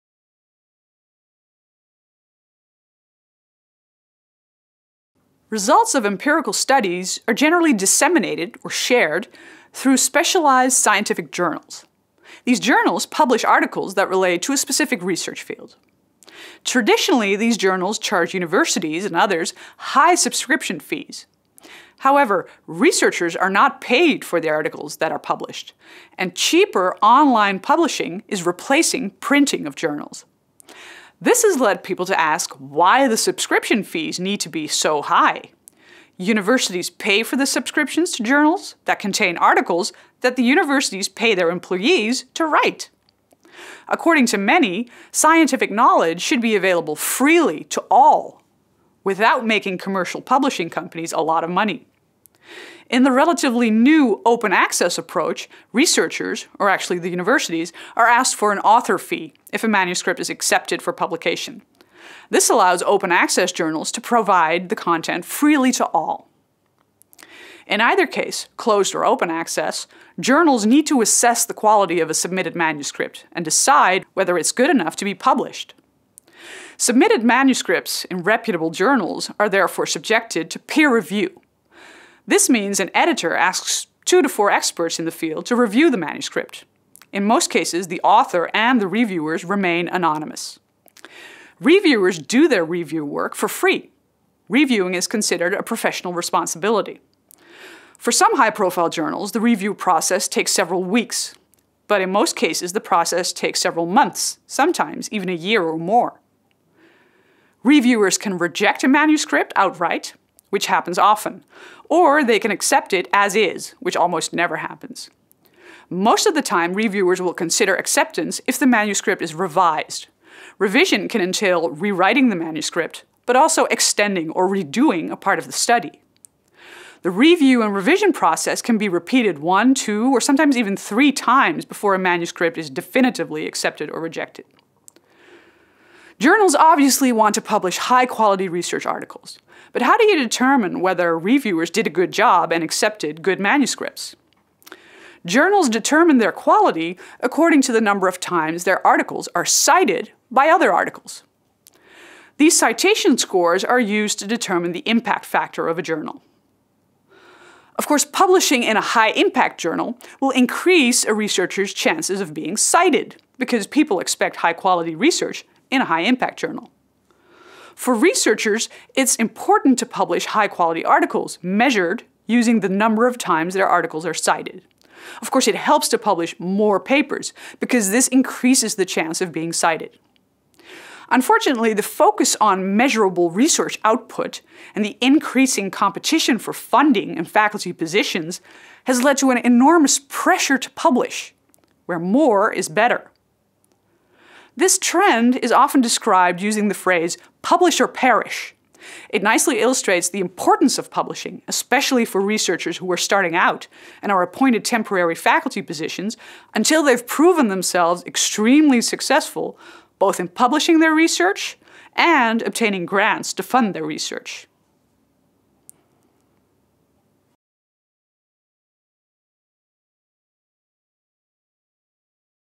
6.7 Peer review process | Quantitative methods | Practice, Ethics and Integrity | UvA
This video explains the process of peer review in academic journals. You also learn about open access publishing and the increasing pressure to publish.